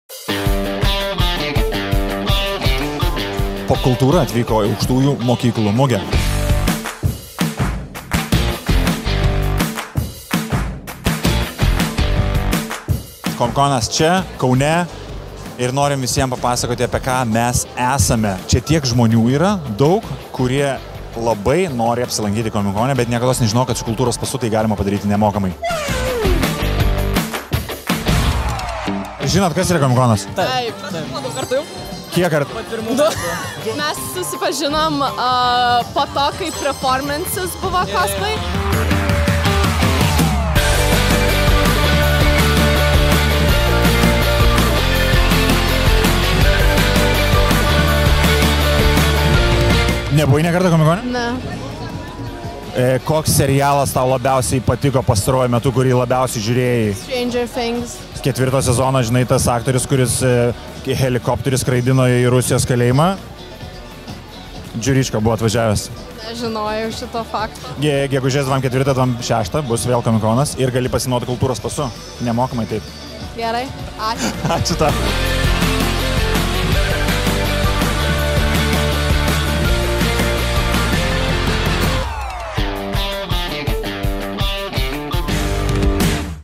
Comic Con Baltics Aukštųjų mokyklų mugėje
Comic Con’as apsilankė Aukštųjų mokyklų mugėje, Kaune! 🎓

Moksleiviai čia galėjo išbandyti savo įgūdžius žaisdami Just Dance, Pacman, įsiamžinti su superherojais bei sužinoti, kaip jie gali apsilankyti Comic Con’e pasinaudodami Kultūros pasu. 😍

Sužinok apie Kultūros pasą ir tu, bei būk su mumis smagiausiame metų savaitgalyje gegužės 24-26 d., LITEXPO, Vilniuje.